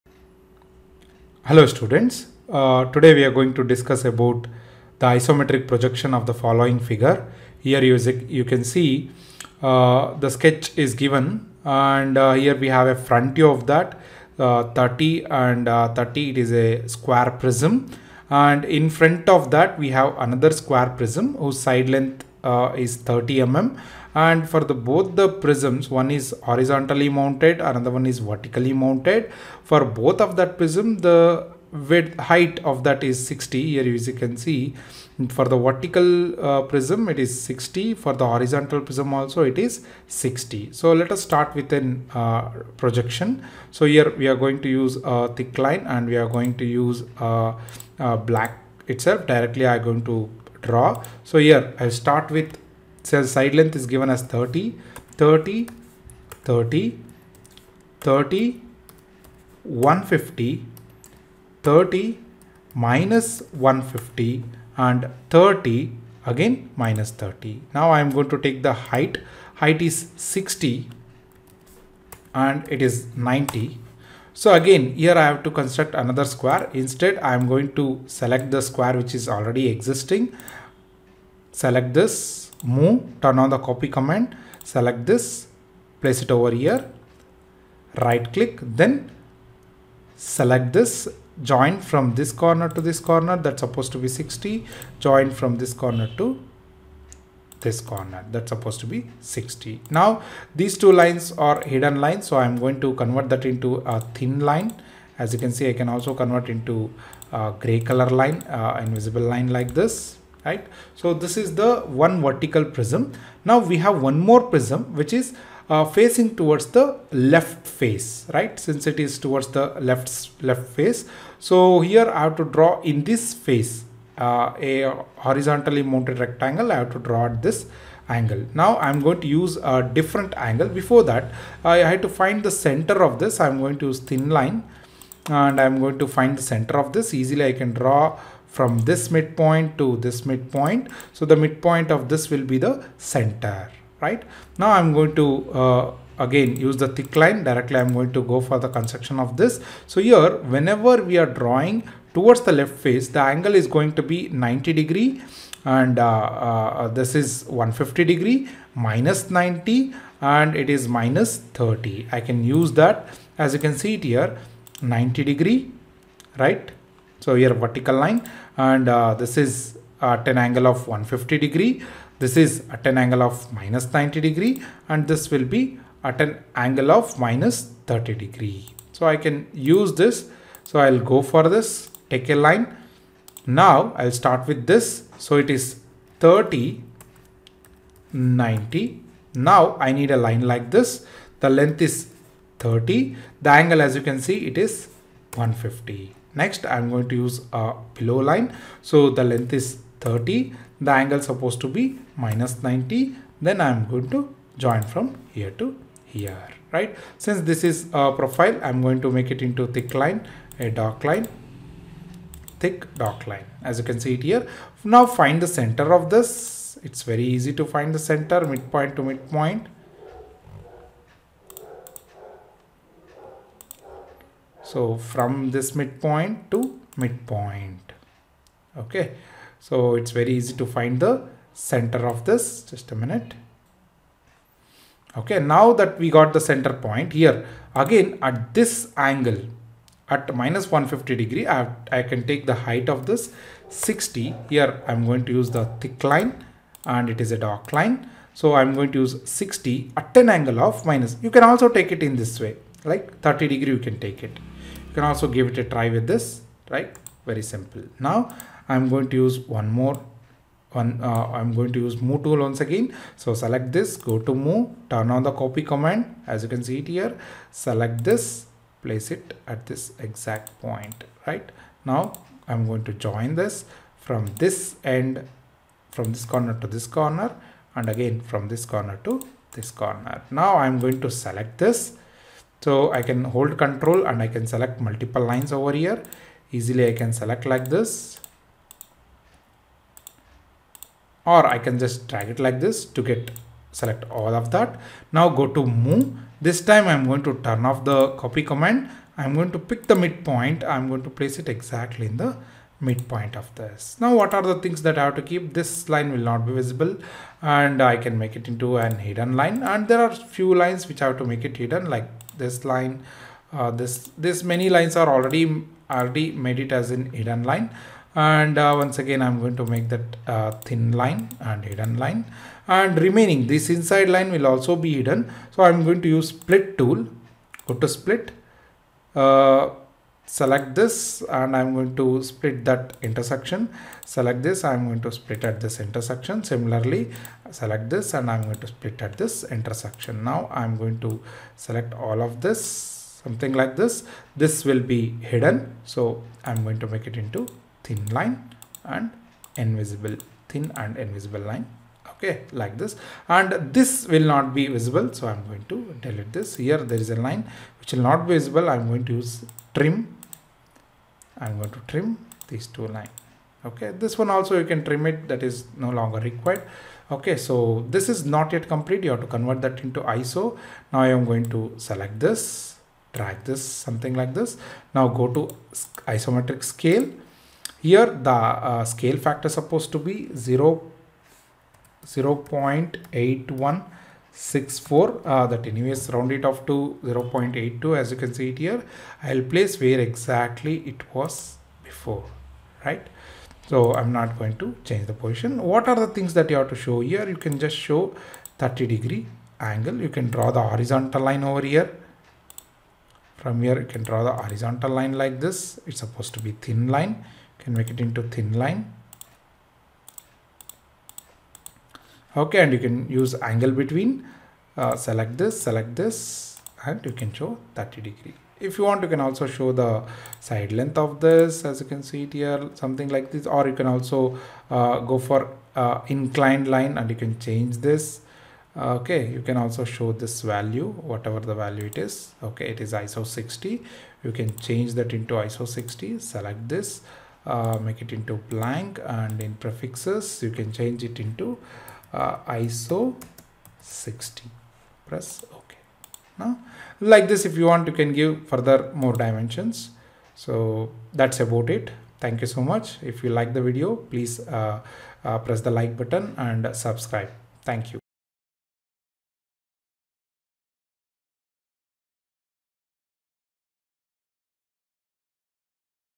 ISOMETRIC PROJECTION PROBLEM 27
DRAW THE ISOMETRIC PROJECTION OF THE FOLLOWING SOLID AS SHOWN IN THE FIGURE.